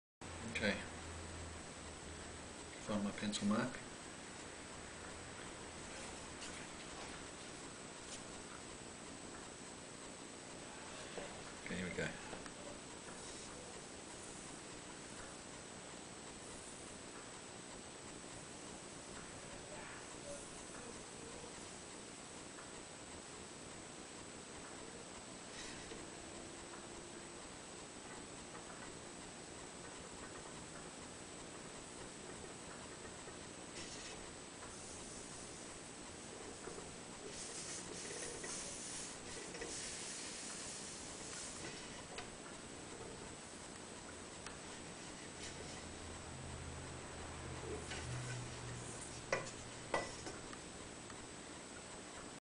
Bending double bass linings
Matthew bending double bass rib linings.
These are 4mm x 25mm strips of timber, wetted and bent on a hot steel bending iron.